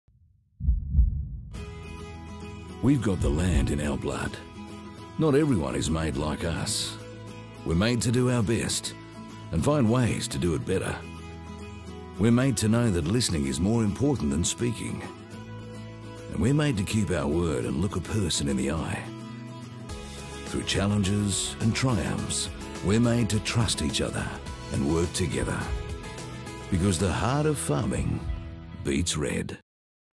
The heart of farming beats red – Case IH Australia TV commercial
We’ve got the land in our blood
Not everyone is made like us
We’re made to do our best
And find ways of doing it better
We’re made to know that listening
is more important than speaking
We’re made to keep our word
and look a person in the eye
Through challenges and triumphs
We’re made to trust each other
and work together
Because the heart of farming beats red